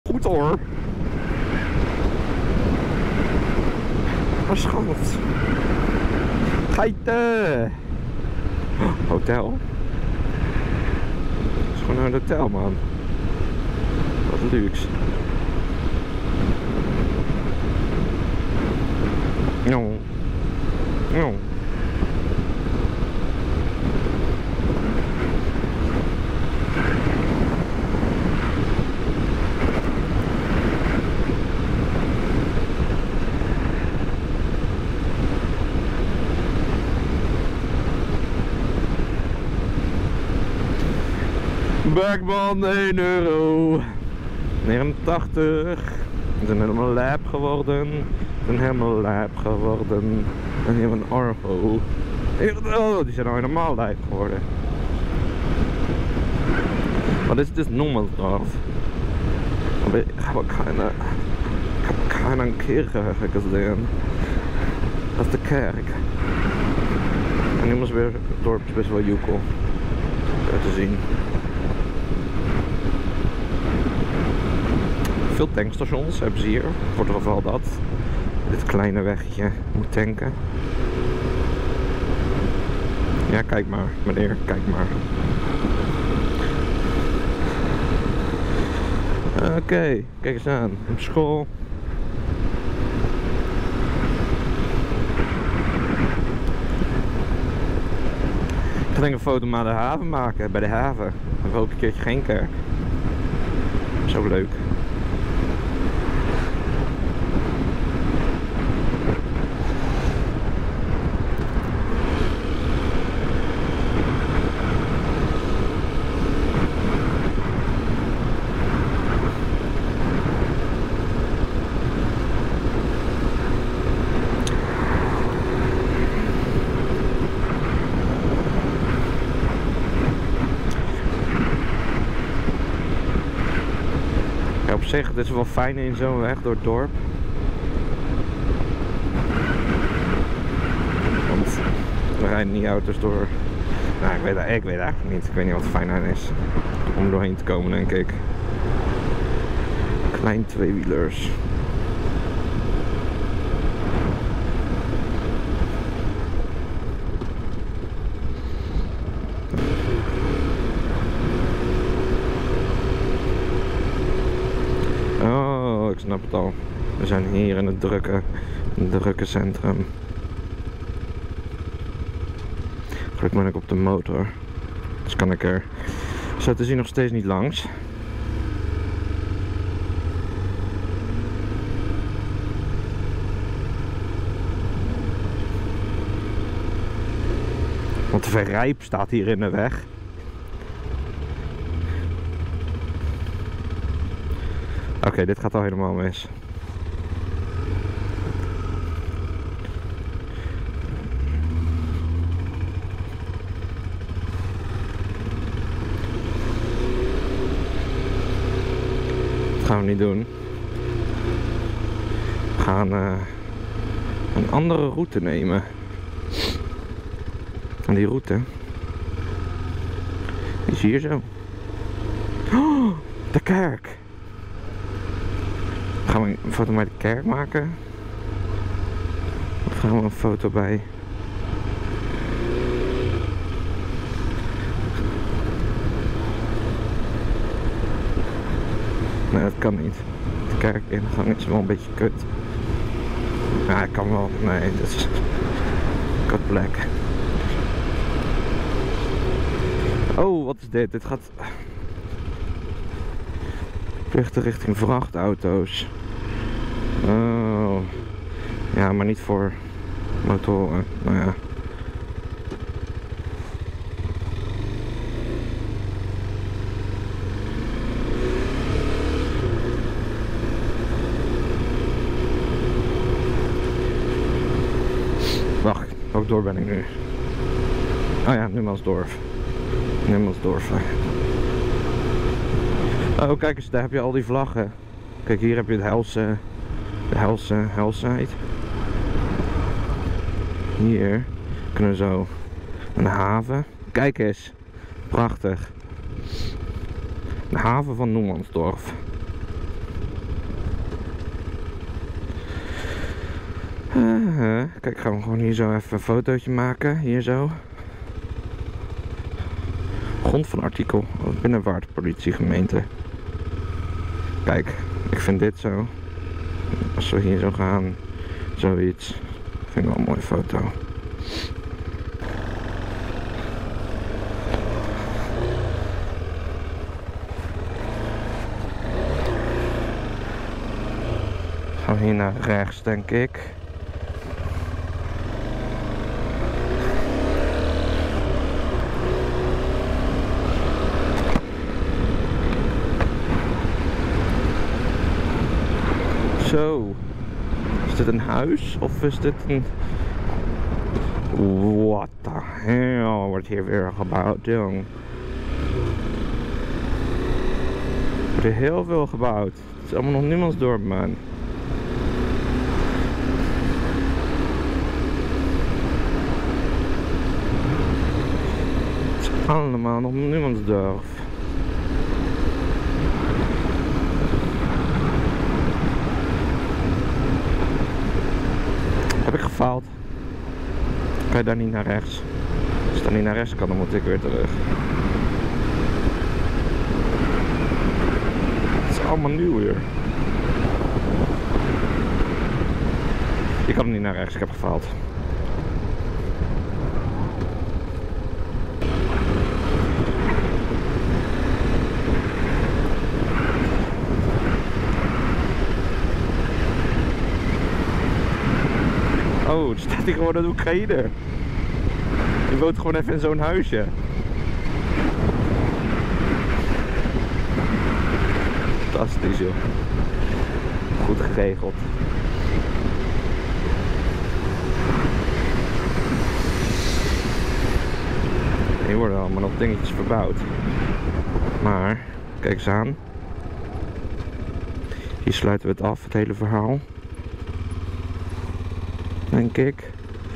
Numansdorp, The Netherlands | 2024 | Winter
Riding on Erbert through Numansdorp in The Netherlands during the 2024 winter.

Recording:
GoPro Hero 12 Black
Rode Lavalier GO Microphone
Media Mod
H1n Zoom Recorder